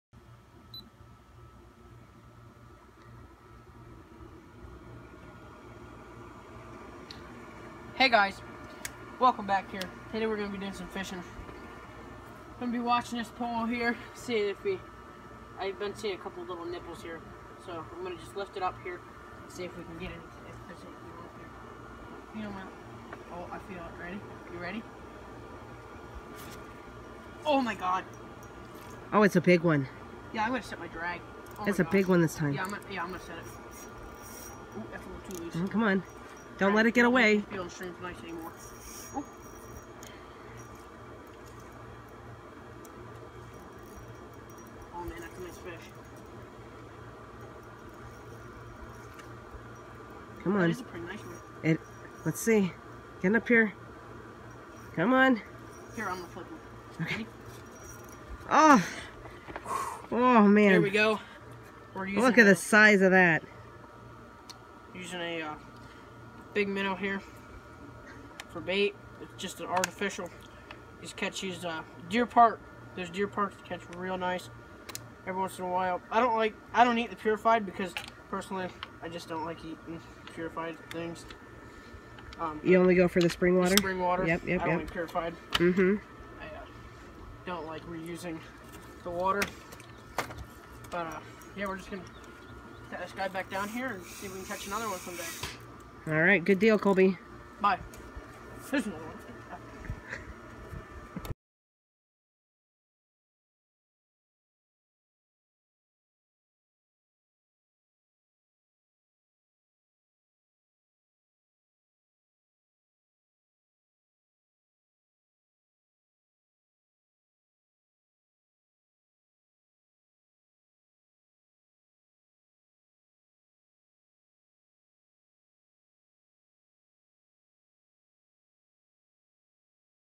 Redneck Sportfishing - Sport Fishing with Colby!
Redneck Sportfishing - Sport Fishing with Colby!   Colby shows you how to catch that MONSTER fish!